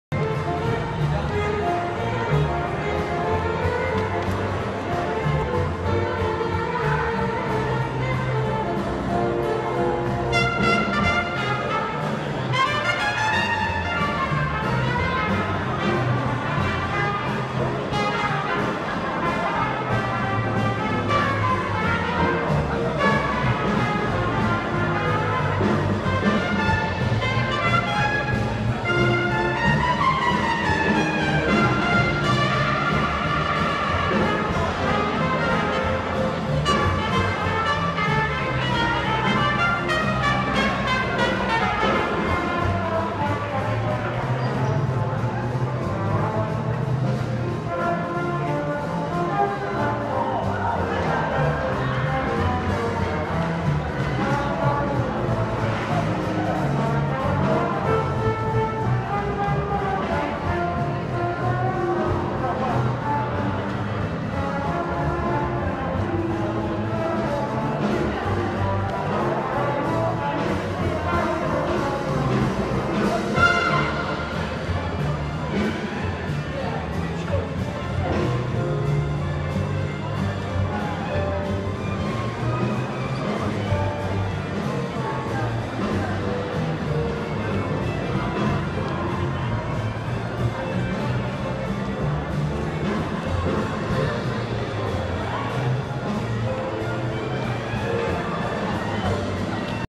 Liverpool Street Station
Pessoal dançando em Londres proximo a estação de Liverpool Street.

Just at the Spitalfields Market located at Liverpool Street Station in London. Good vibes and good energy.